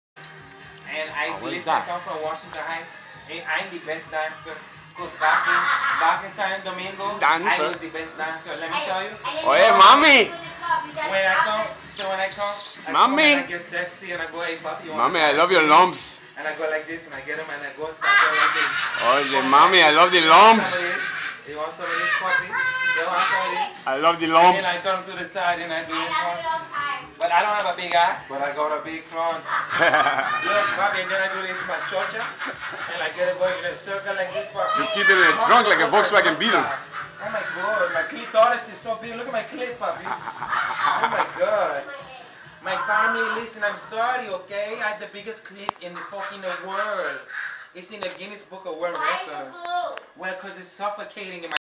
Belly Twerking
Ooops Chis fell into Alex's closet. And becomes a lovely Latina, who can twerk her belly like a champ